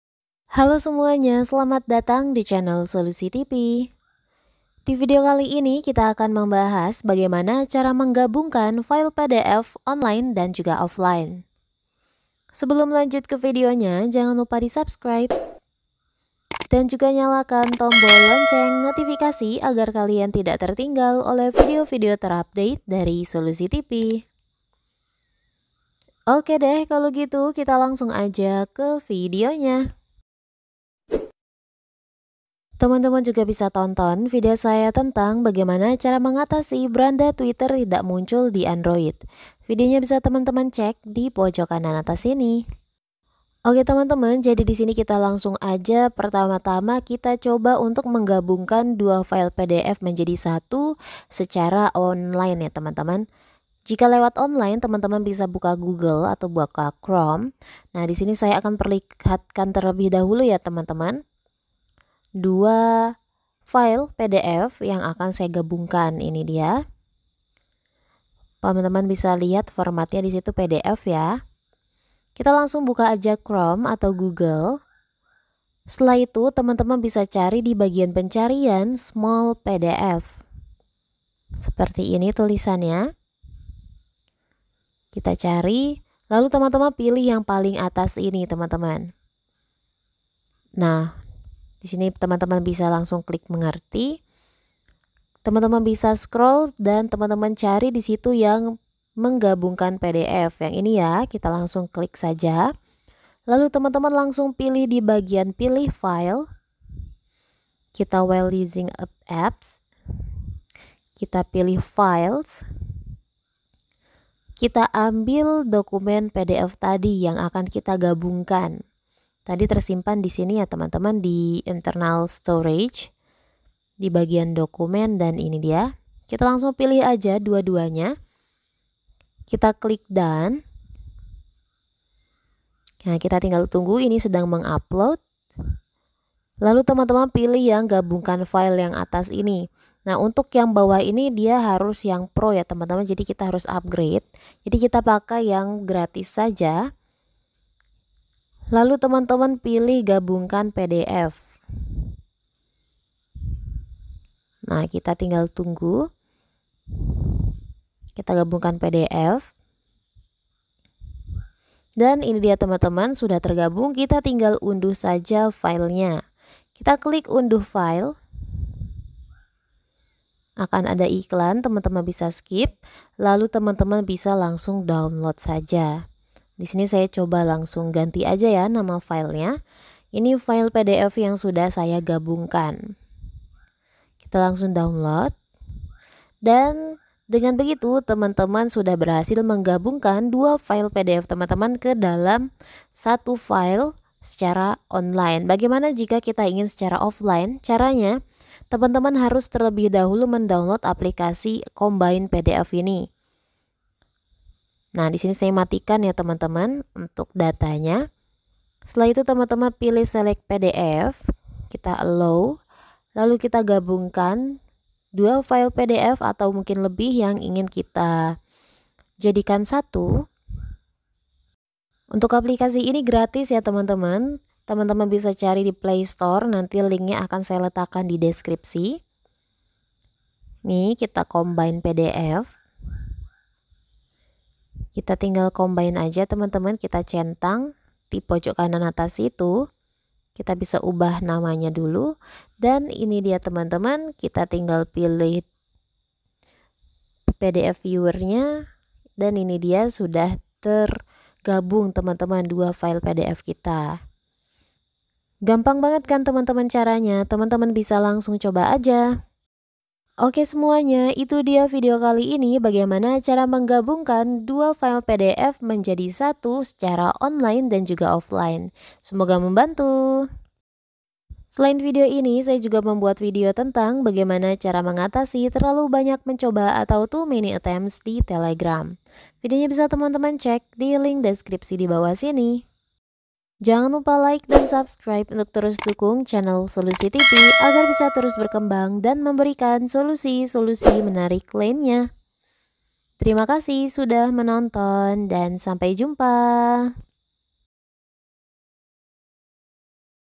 Cara Menggabungkan File PDF ( Online & Offline )
Cara Menggabungkan File Pdf Offline :
-Instal terlebih dahulu software Adobe Reader, walaupun biasanya software tersebut sudah terdapat pada setiap laptop.
-Buka Adobe Reder kemudian pilih opsi Add Files.
-Carilah file yang akan kamu gabungkan, kemudian pilih opsi combine.

Selain gabung PDF menggunakan android dan software offline, Anda juga bisa menggunakan situs online untuk menggabungkan file PDF menjadi satu.